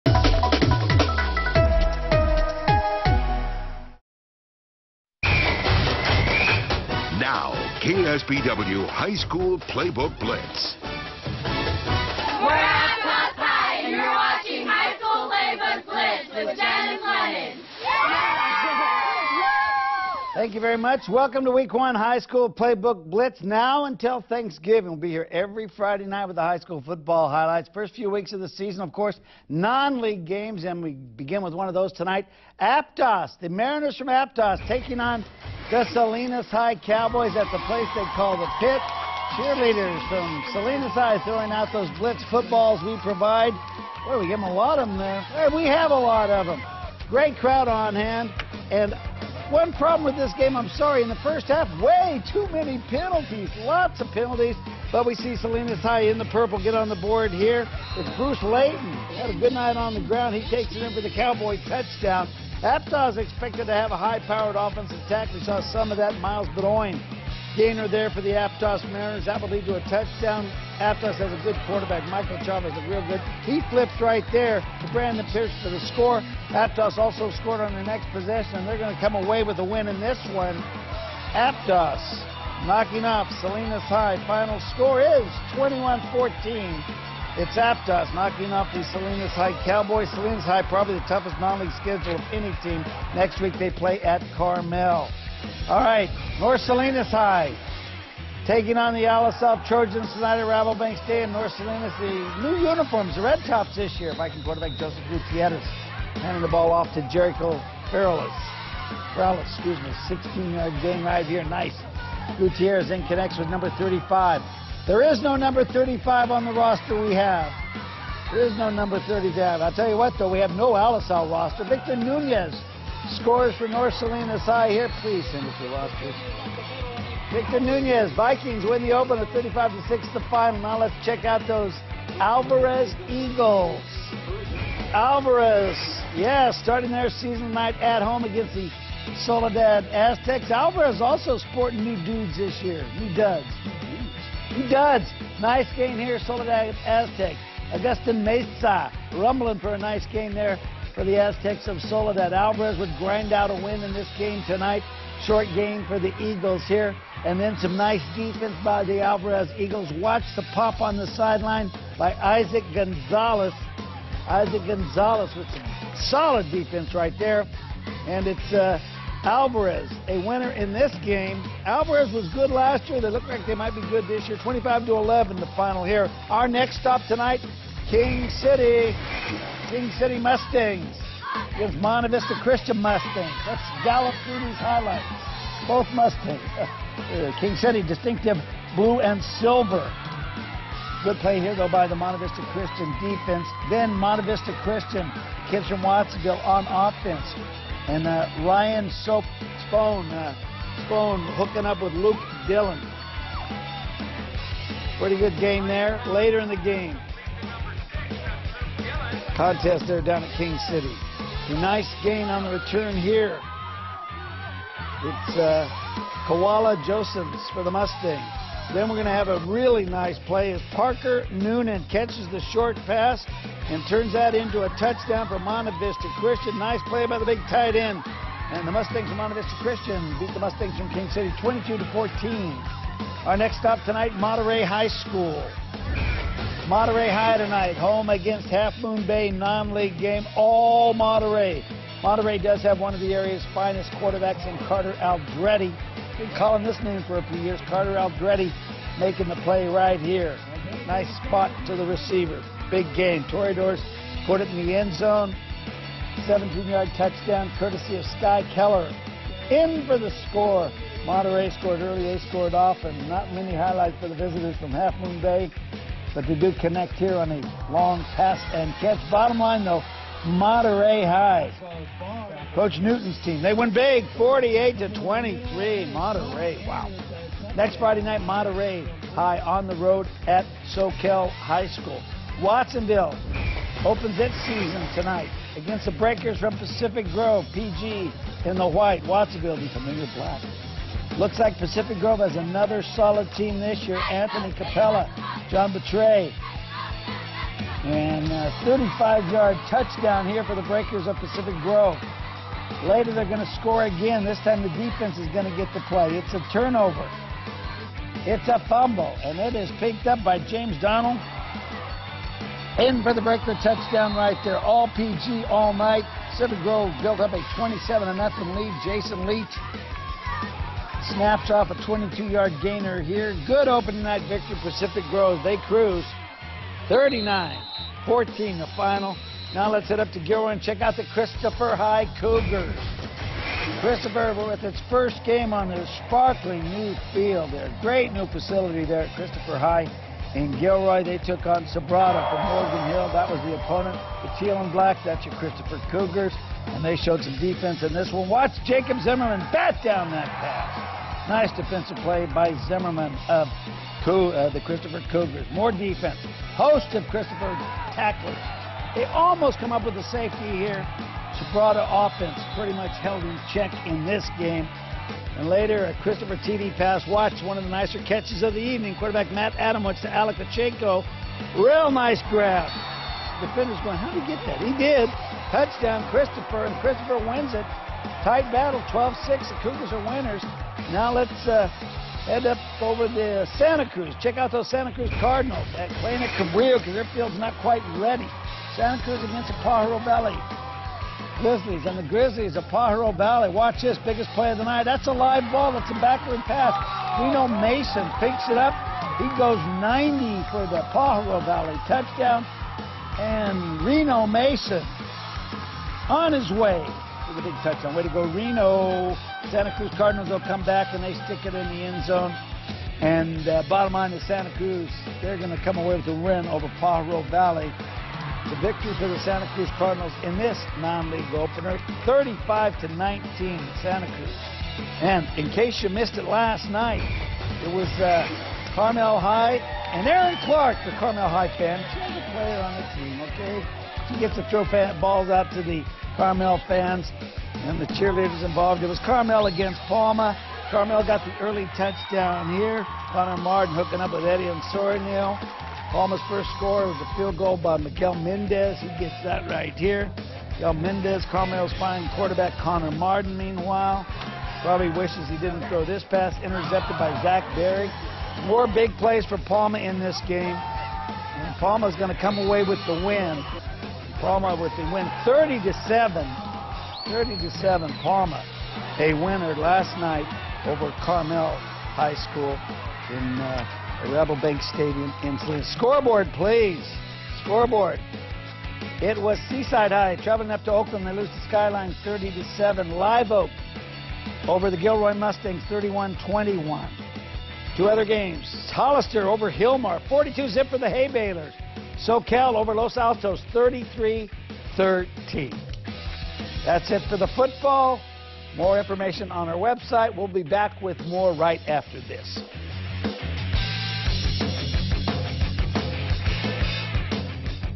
High School Playbook Blitz 2014 Edition: Week 1
Dennis Lehnen breaks down Week 1 of Central Coast high school football.